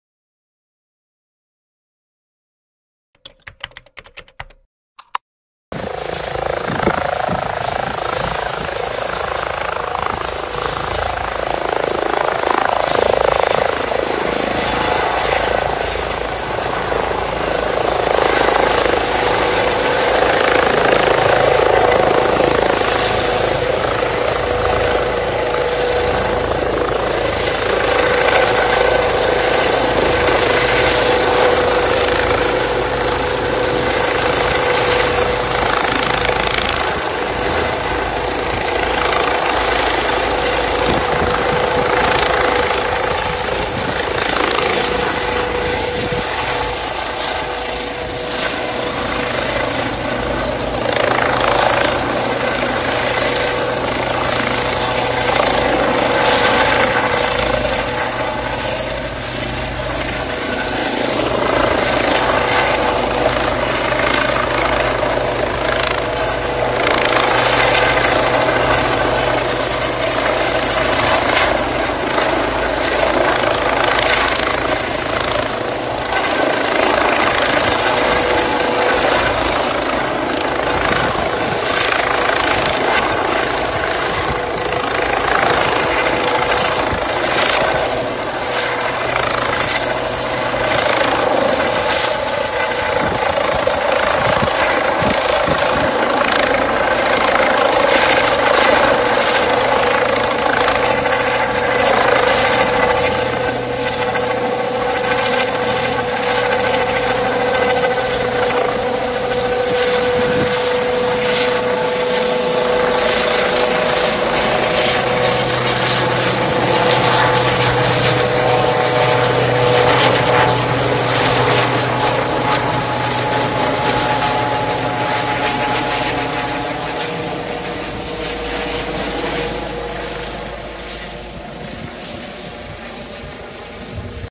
Wybuch beczki w Koszalinie! Mężczyzna w krytycznym stanie.
W czwartek, 30-go lipca 2020 roku przy ulicy Topolowej w Koszalinie doszło do wypadku.